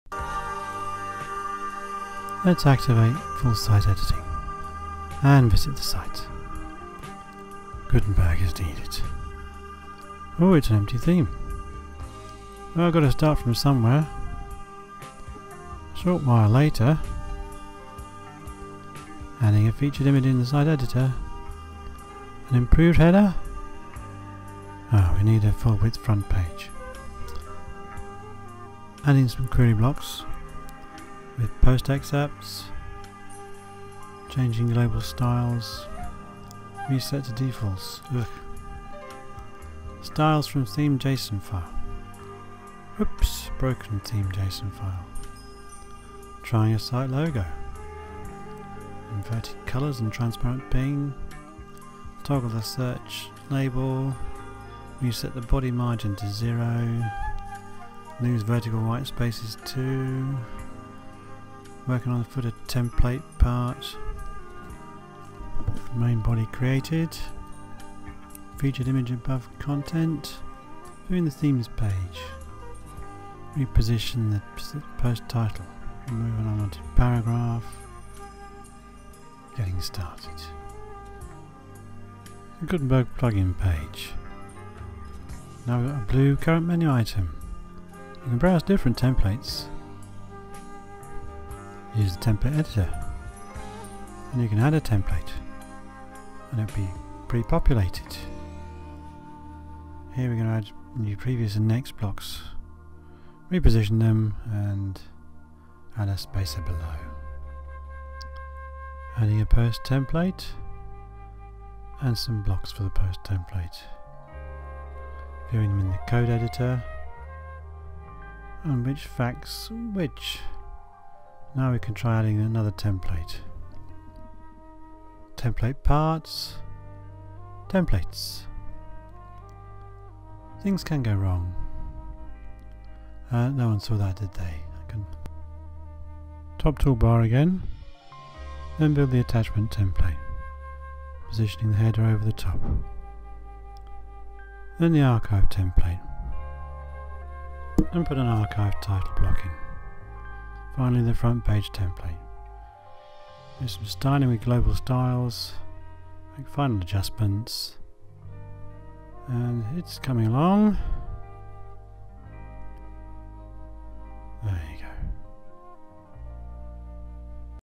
ThisIs... A guide to FSE - narrated snapshots of the WordPress Full Site Editing experience
The WordPress Site Editor is currently being developed in the Gutenberg plugin. In this slideshow I demonstate the "ThisIs..." theme being construcuted from scratch. There are 55 screenshots each lasting 3 seconds. Not enough time for a proper narrative, but you should get the gist.

You'll see things that work and things that didn't quite work properly.
You won't see me writing any code, though I did need to edit style.css a couple of times for things that I couldn't put into the Global Styles file ( currently called experimental-theme.json ).

The Site Editor is still being developed. The screenshots were taken using WordPress 5.7.1 and Gutenberg 10.4.1. Now that Gutenberg 10.5.3 has been released some of them are already out of date!

The theme is available from GitHub

This video is part of a website I'm creating in order to give a presentation entitled "A guide to Full Site Editing, blocks and themes".

The presentation's first airing will be at the WordPress Portsmouth Online Meetup on the 19th of May 2021.